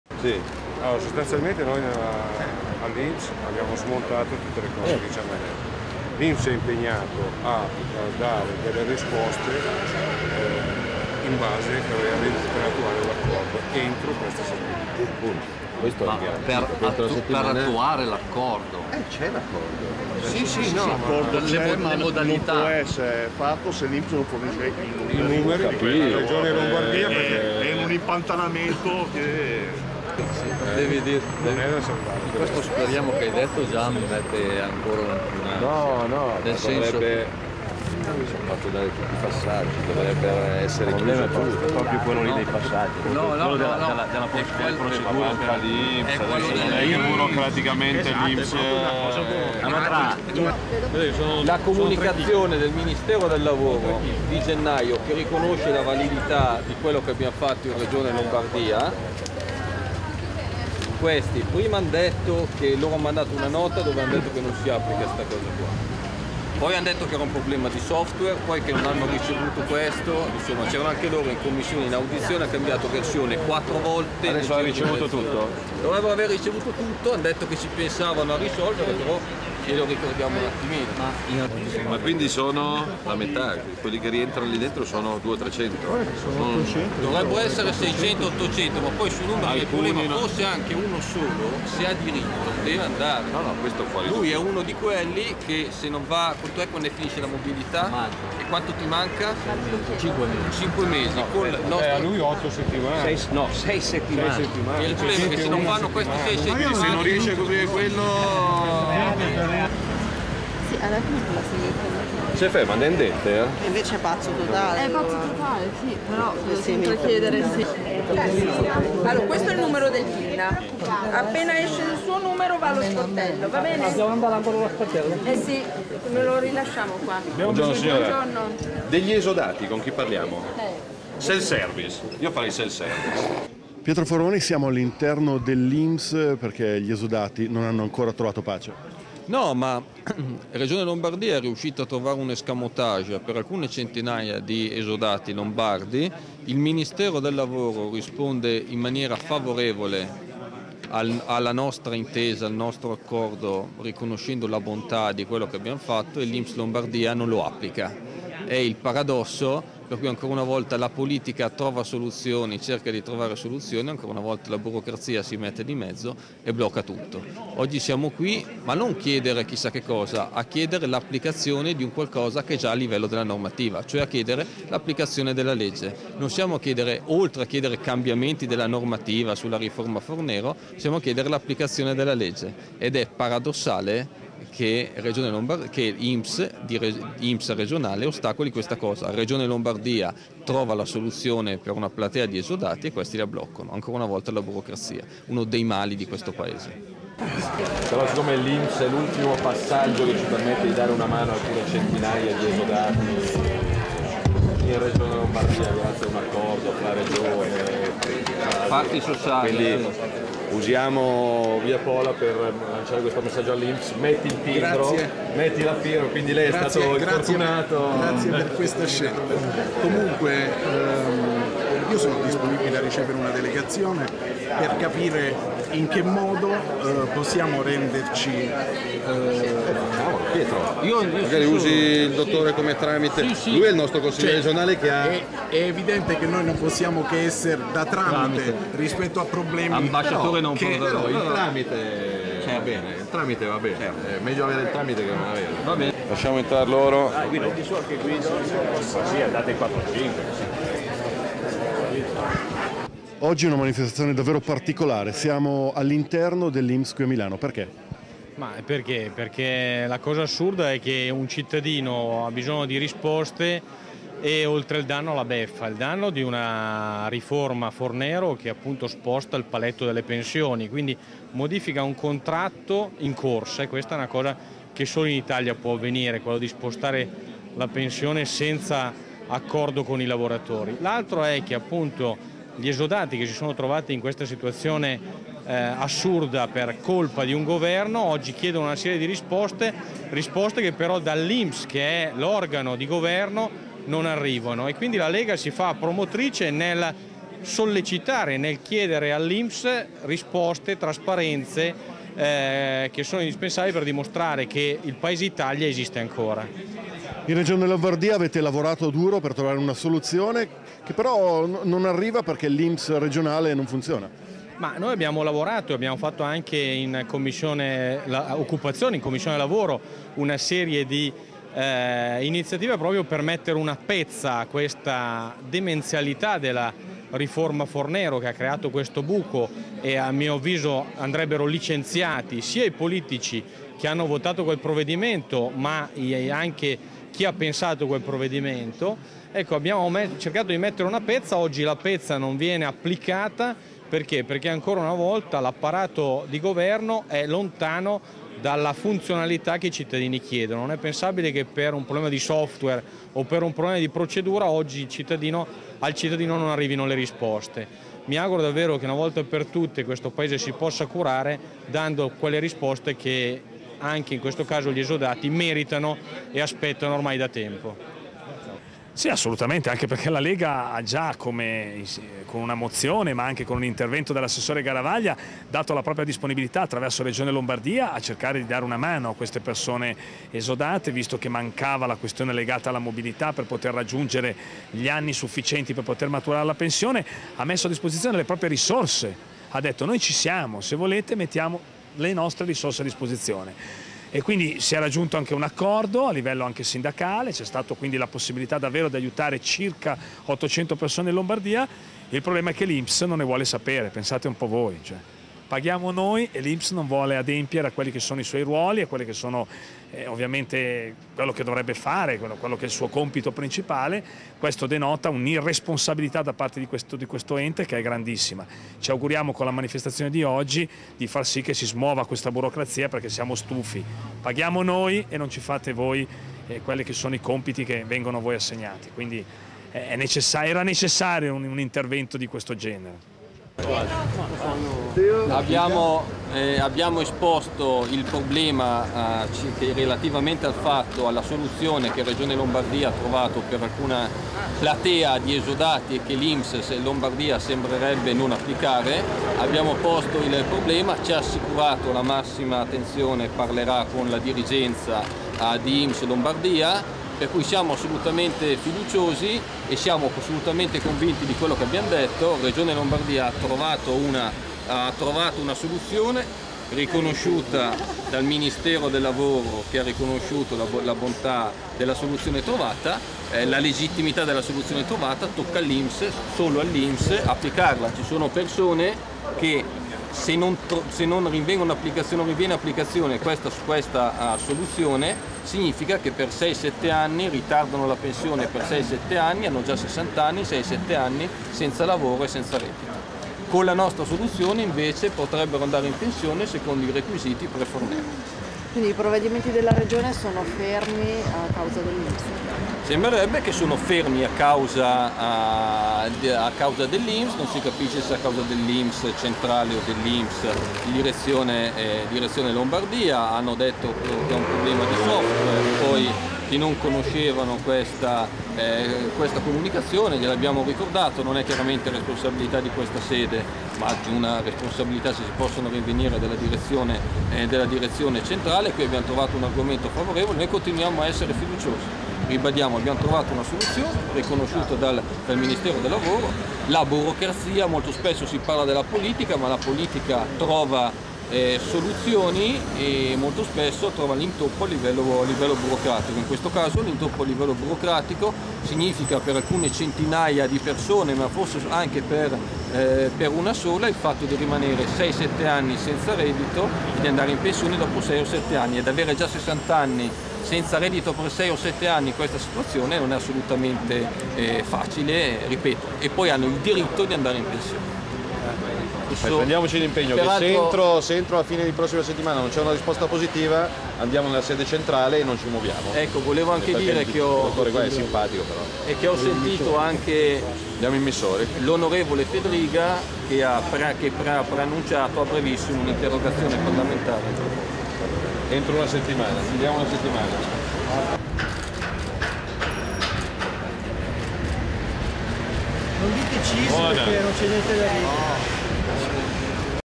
LEGA NORD - MANIFESTAZIONE DAVANTI INPS - ESODATI - MATTEO SALVINI E CONSIGLIERI REGIONALI
LEGA NORD - MANIFESTAZIONE DAVANTI  INPS - ESODATI - MATTEO SALVINI E CONSIGLIERI REGIONALI - sede INPS DI VIA POLA